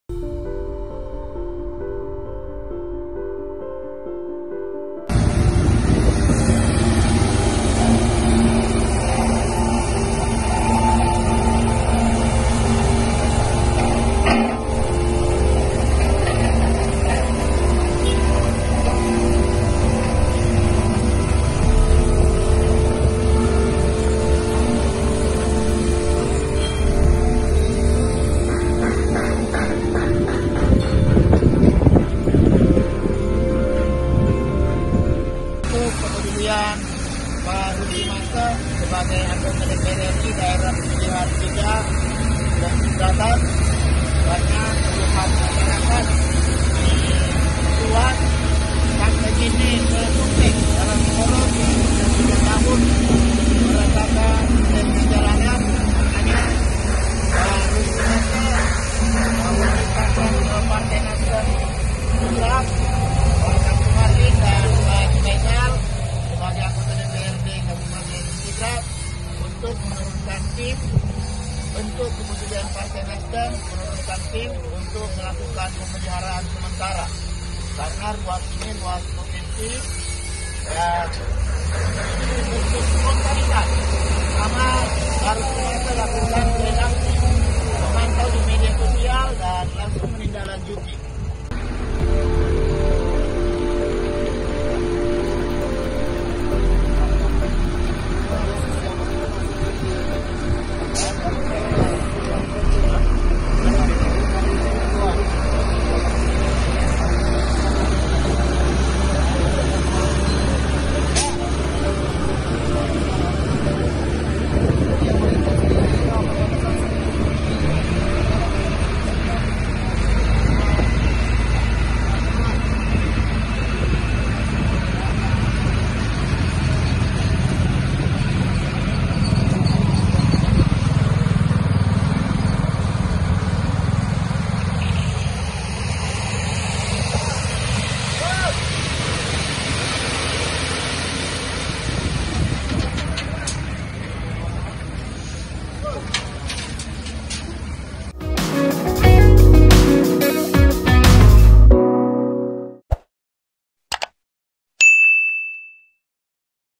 NasDem Sulsel Turunkan Alat Perbaiki Jalan Provinsi Sidrap – Soppeng
Partai NasDem Sulawesi Selatan kerahkan alat berat untuk melakukan perbaikan Jalan Provinsi penghubung Kabupaten Sidrap-Kabupaten Soppeng, Sulsel.

Sebelumnya, NasDem Sulsel juga mengerahkan alat berat untuk melakukan perbaikan jalan di Kecamatan Rampi, Kabupaten Luwu Utara.

Sekretaris DPW Partai NasDem Sulsel, Syaharuddin Alrif mengatakan bahwa, ini merupakan intruksi langsung dari RMS kepada NasDem Sidrap untuk lakukan pengerjaan perataan jalanan ruas Sidrap – Soppeng.

“Ini bentuk kepedulian NasDem sulsel dan pak RMS yg merupakan anggota DPR RI dapil 3 dan pak Syaharuddin alrif Anggota DPRD sulsel dapil Sidrap,” tutur Syahar sapaan akrabnya, Senin (23/5/2022).
Video terkini dan terbaru..